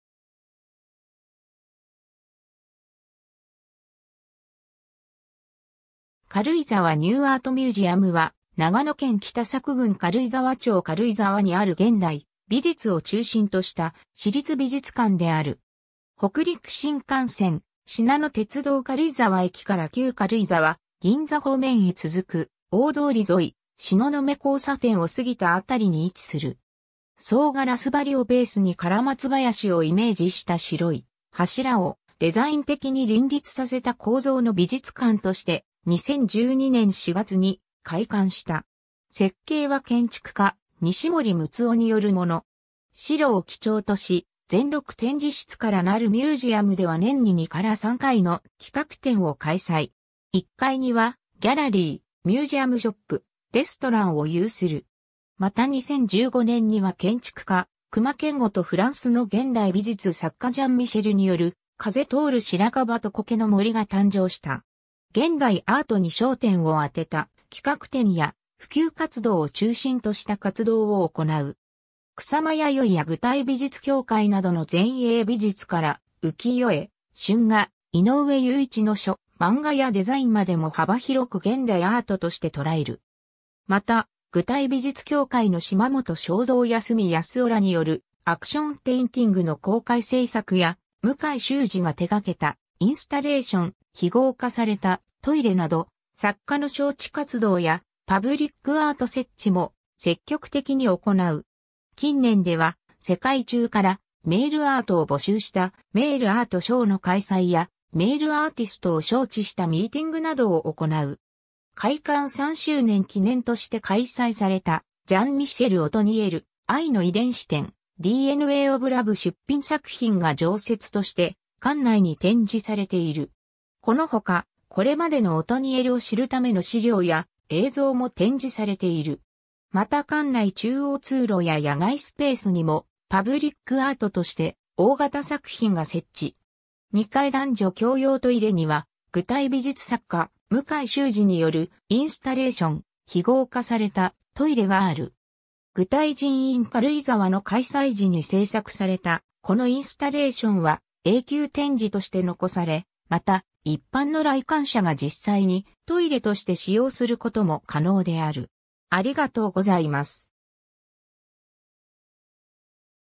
軽井沢ニューアートミュージアム
軽井沢ニューアートミュージアム（かるいざわニューアートミュージアム、英: KARUIZAWA NEW ART MUSEUM）は、長野県北佐久郡軽井沢町軽井沢にある現代美術を中心とした私立美術館である。
 北陸新幹線・しなの鉄道軽井沢駅から旧軽井沢銀座方面へ続く大通り沿い、東雲交差点を過ぎたあたりに位置する。
総ガラス張りをベースにカラマツ林をイメージした白い柱をデザイン的に林立させた構造の美術館として、2012年4月に開館した。
 設計は建築家西森陸雄によるもの。
 白を基調とし、全6展示室からなるミュージアムでは年に2～3回の企画展を開催。
1階にはギャラリー、ミュージアムショップ、レストランを有する。
 また2015年には建築家隈研吾とフランスの現代美術作家ジャン＝ミシェルオトニエルによる『風通る白樺と苔の森（チャペル）』が誕生した。
 現代アートに焦点を当てた企画展や普及活動を中心とした活動を行う。
草間彌生や具体美術協会などの前衛美術から、浮世絵・春画、井上有一の書、漫画やデザインまでも幅広く現代アートとして捉える。
 また具体美術協会の嶋本昭三や鷲見康夫らによるアクションペインティングの公開制作（パフォーマンス）や、向井修二が手掛けたインスタレーション『記号化されたトイレ』（2014年）など作家の招致活動やパブリックアート設置も積極的に行う。
 近年では世界中からメールアートを募集した「Mail Art Show」（2016年）の開催や、メールアーティストを招致したミーティングなどを行う。
 開館3周年記念として開催されたジャン＝ミシェル オトニエル「愛の遺伝子展 〜DNA of Love〜」出品作品が常設として館内に展示されている。
(第4展示室)この他、これまでのオトニエルを知るための資料や映像も展示されている。
また館内中央通路や野外スペースにもパブリックアートとして大型作品が設置。
 2階男女共用トイレには具体美術作家、向井修二によるインスタレーション『記号化されたトイレ』がある。
 「具体人 in Karuizawa」の開催時に制作されたこのインスタレーションは永久展示として残され、また一般の来館者が実際にトイレとして使用することも可能である。